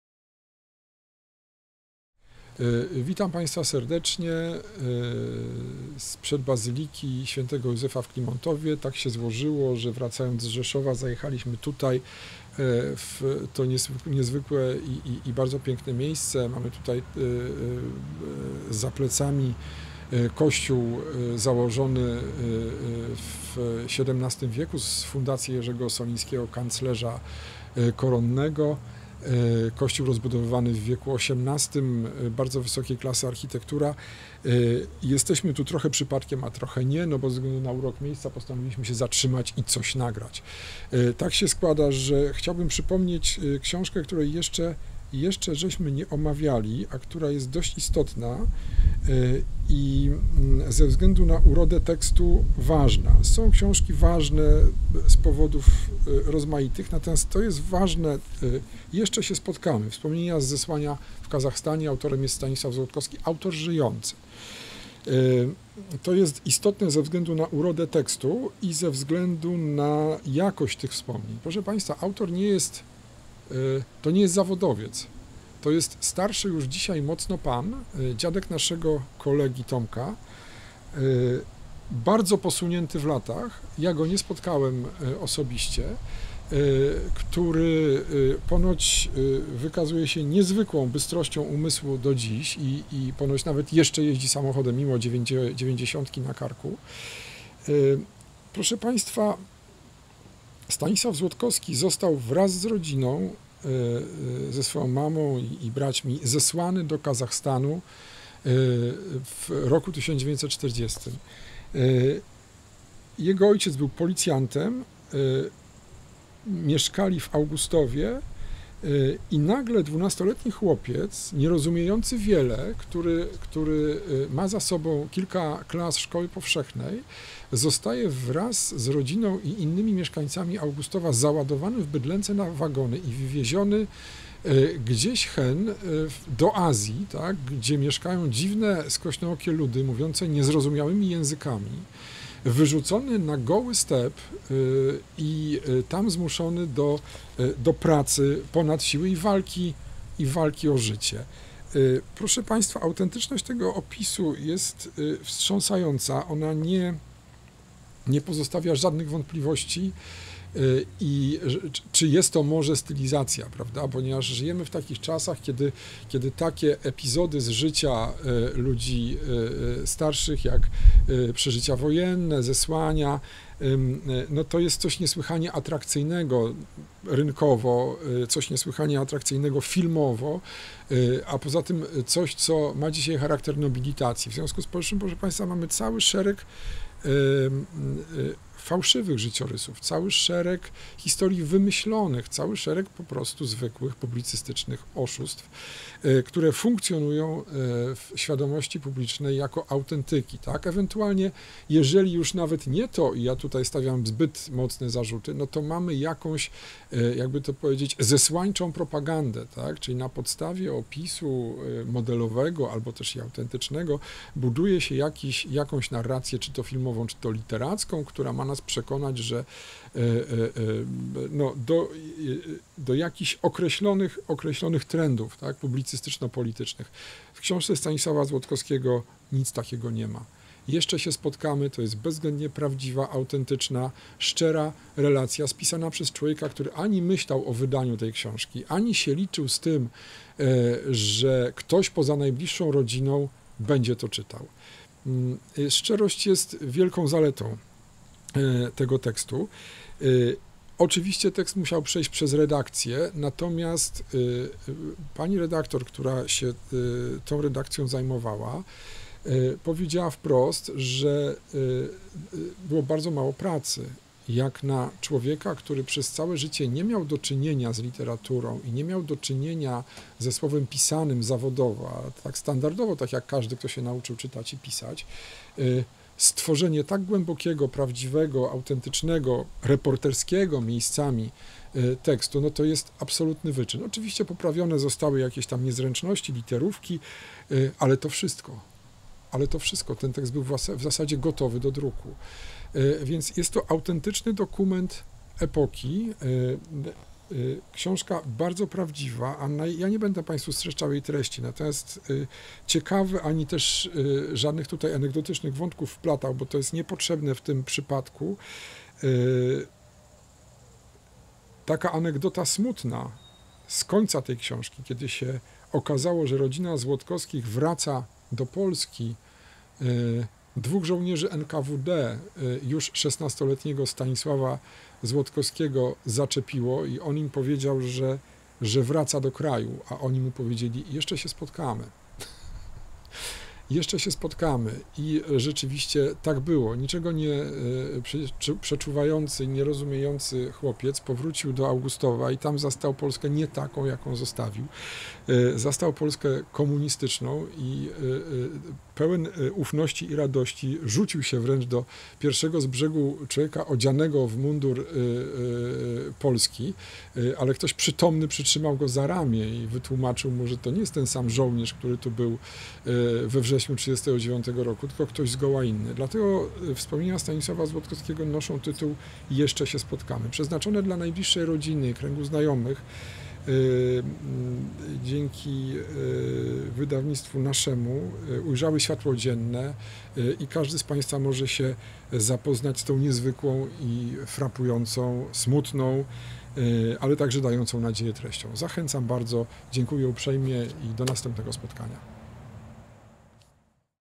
Gabriel Maciejewski o książce: Stanisław Złotkowski - Jeszcze się spotkamy
"W Augustowie rządzili już Sowieci. Naszą rodzinną ziemię augustowską nazywali Zachodnią Białorusią. Od razu po naszym przyjeździe z Lubieszowa wujek Romanowski oznajmił nam, że do naszego domu przychodzili enkawudziści i pytali o ojca. Wujek powiedział im, że pojechał po rodzinę na wieś, nie wie, gdzie, ale niedługo powinien przyjechać. Przykazali, żeby po powrocie zgłosił się do tutejszego biura NKWD. Wiadomość ta wszystkich nas bardzo zatrwożyła, gdyż wezwanie do stawienia się w NKWD szefa przedwojennej policji powiatowej mogło oznaczać tylko jedno: więzienie, łagry, katorgę, a nawet śmierć przez rozstrzelanie. Ojciec doskonale o tym wiedział i rozmyślał nad tym, jak ma postąpić, ale właściwie dużego wyboru nie miał. Co życzliwsi sąsiedzi radzili, aby uciekał. Ale dokąd? Cała Polska jest już zajęta przez Niemców albo Sowietów. Najbliżej jest Litwa, ale ona również nie jest przyjazna dla Polski. Bezpiecznego schronienia nigdzie już nie ma. To, co teraz dzieje się w Polsce to tylko początek wielkiej wojny światowej, która obejmie całą Europę, a może nawet i cały świat – tak mówił ojciec. Uważał, że zawarty pakt trójstronny, czyli oś Berlin-Rzym-Tokio nie wróży, aby na Polsce wszystko się zakończyło. Teraz doszedł jeszcze Związek Radziecki. Wszyscy oni pragną panować nad całym światem. Faszyzm z jednej strony, a komunizm z drugiej. Jesteśmy wobec tych wydarzeń bezsilni." - (Fragment tekstu)
Nagranie siłami "ekipy technicznej" księgarni Foto-Mag w Warszawie, al.KEN 85 lokal 5a